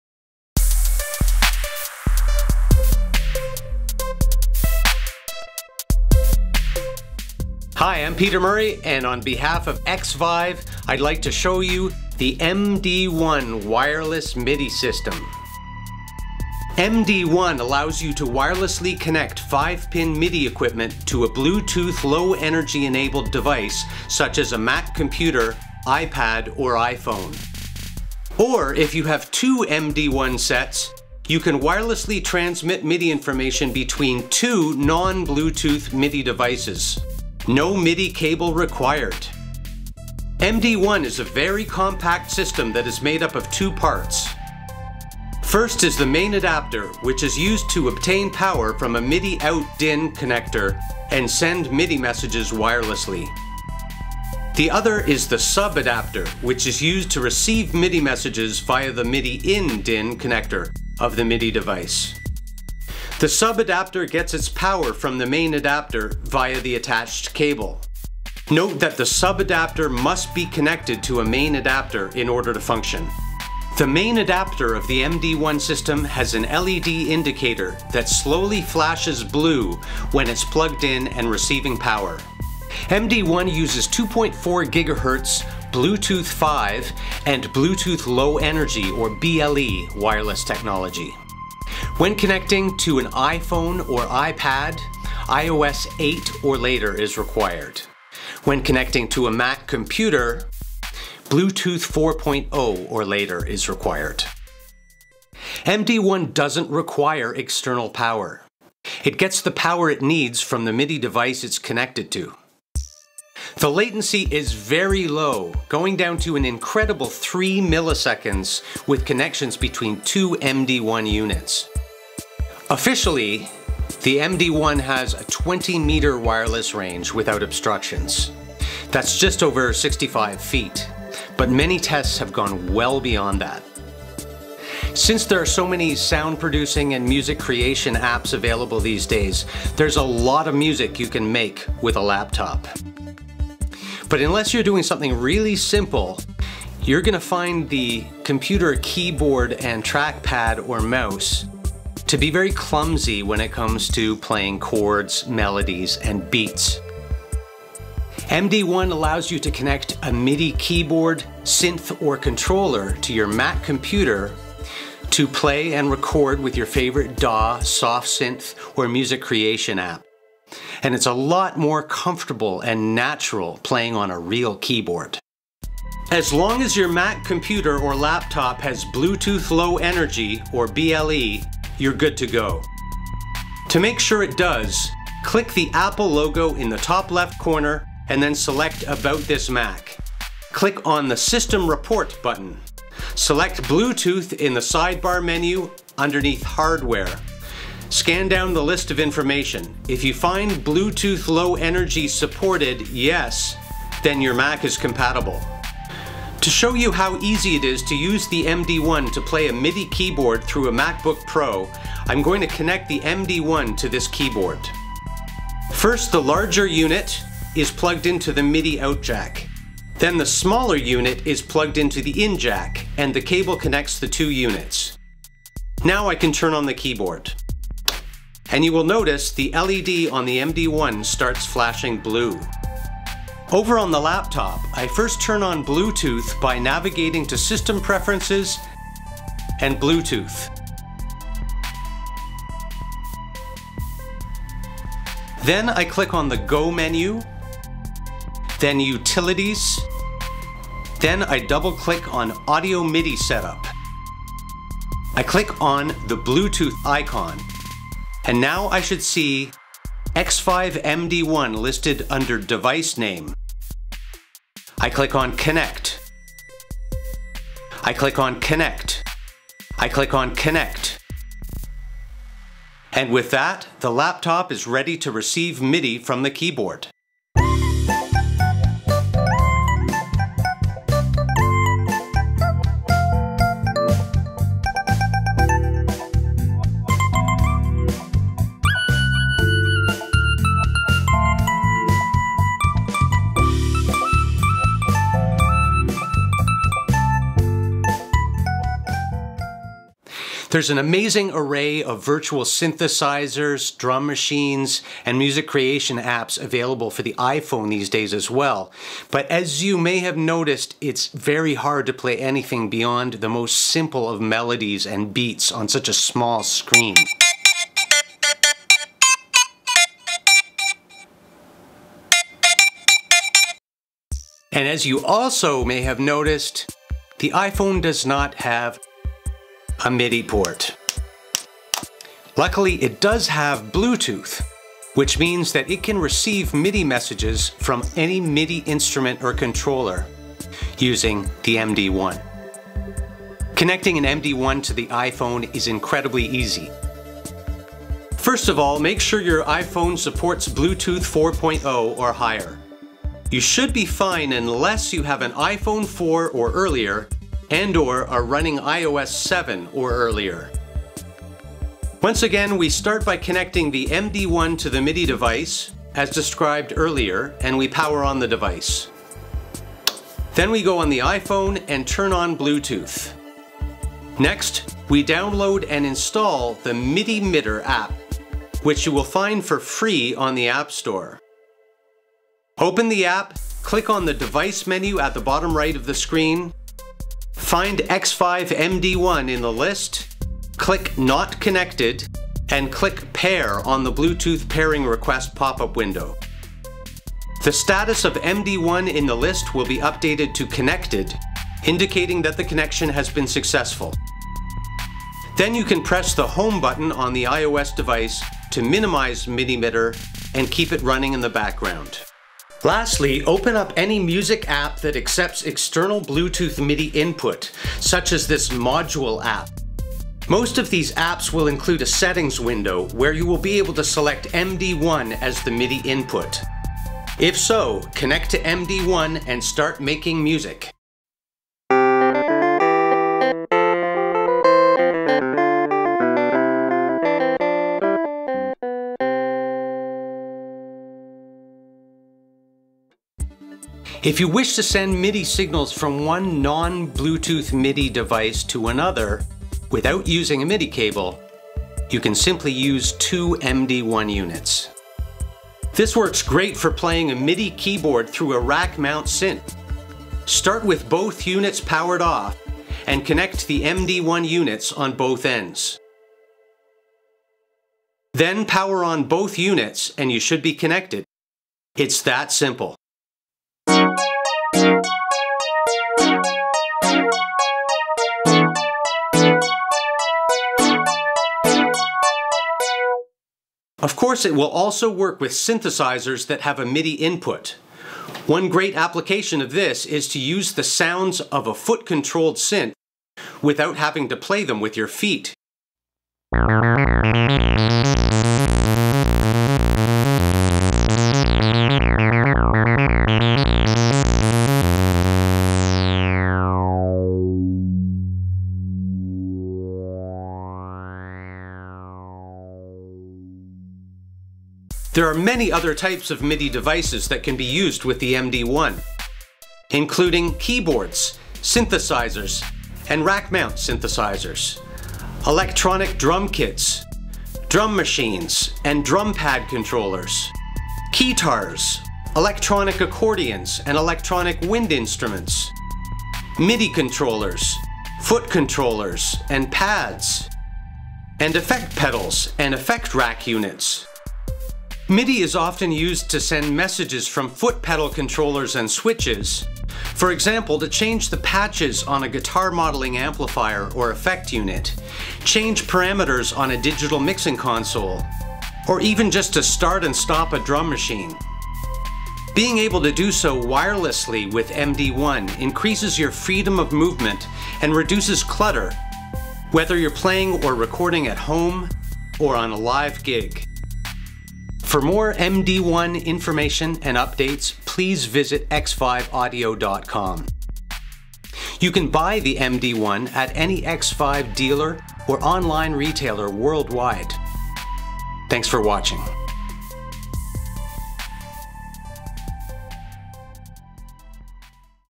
MD1 Wireless MIDI System - Official XVIVE Video
MD1 uses 2.4 GHz Bluetooth 5 and Bluetooth Low Energy (BLE) technology to transmit MIDI messages between 5-pin MIDI devices and BLE-enabled Mac computers, Mac laptops, iPads and iPhones. If two MD1 units are used, the system can also transmit MIDI between two non-Bluetooth MIDI devices.

0:00  Introduction
1:22  Technical specs and requirements
3:14  Connecting to a Mac laptop or desktop computer
5:01  Connecting to an iPhone
7:23  Connecting two non-Bluetooth MIDI devices
8:34  MIDI devices and instruments

In addition to eliminating the clutter, limitations and inconvenience of MIDI cables, the super-compact MD1 system makes it possible to connect a non-Bluetooth MIDI keyboard or controller to an iPhone, iPad or Mac computer. This makes it an invaluable tool for music creators using iOS software and apps. MD1 is also compatible with a wide variety of MIDI gear—including keyboards and synthesizers, MIDI controllers and pads, electronic drum sets and drum machines, foot controllers, profiling guitar amplifiers, digital mixing consoles, effect rack units and even some effect pedals.

MD1 doesn’t require a battery or power supply; it gets its power from whatever MIDI OUT port it’s connected to. The system connects quickly and easily; and its latency is as low as 3 milliseconds when transmitting between two MD1 systems. The wireless range is 20 meters (65 feet) without obstructions.

The MD1 Wireless MIDI System is available now from Xvive dealers and online retailers worldwide, and sells for $49.99 USD.